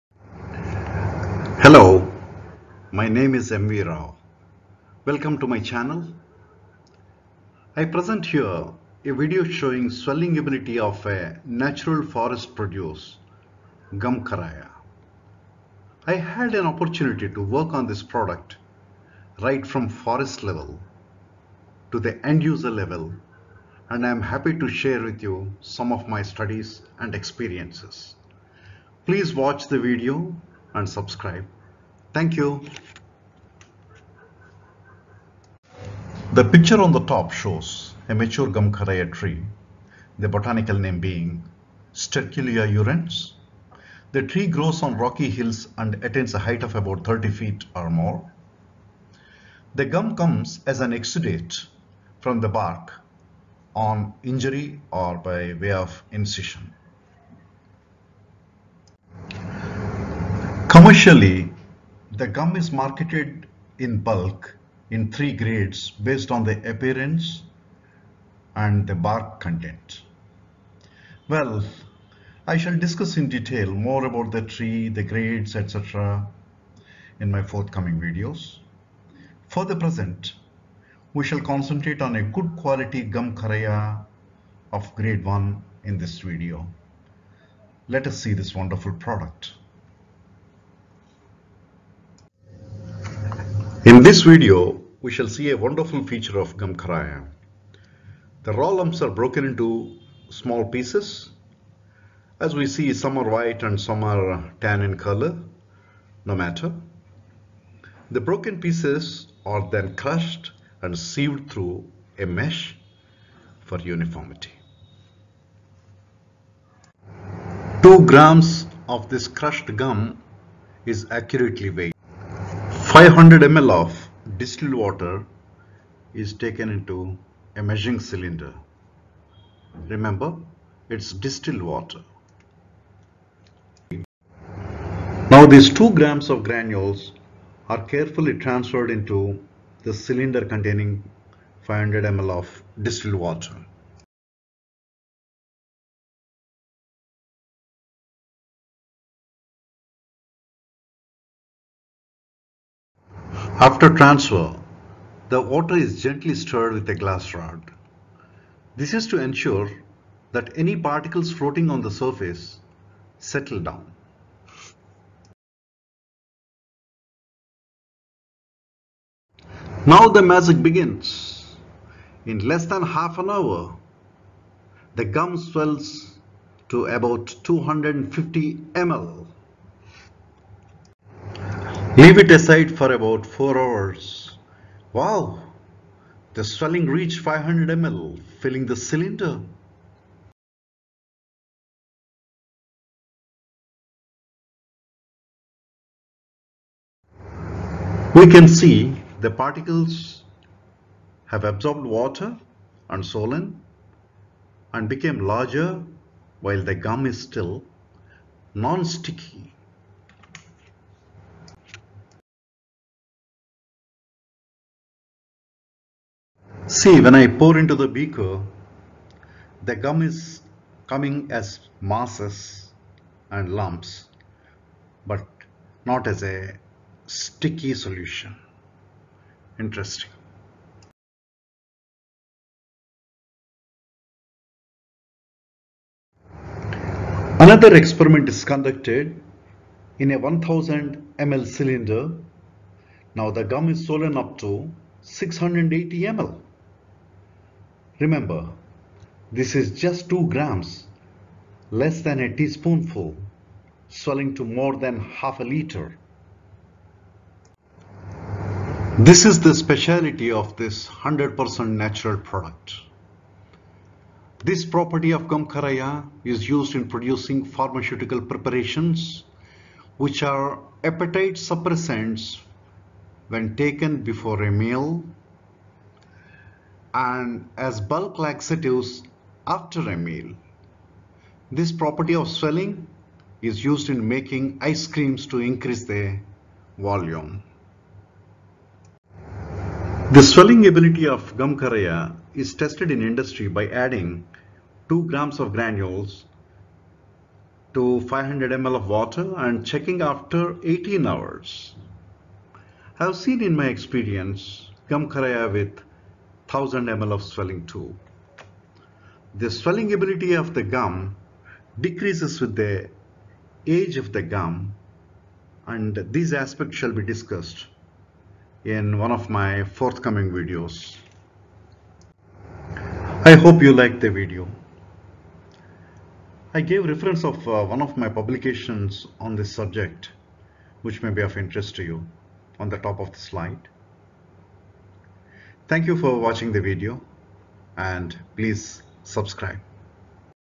GN1. Gums and Natural Products- Karaya - A wonder Gum
ScienceSmilesShorts presents a video on Gum Karaya. The gum is a forest produce and has a great swelling ability in water. Because of the swelling ability it is used in ice creams to build volume, appetite suppressants, bulk laxatives. The method of testing the swelling ability is presented in this video.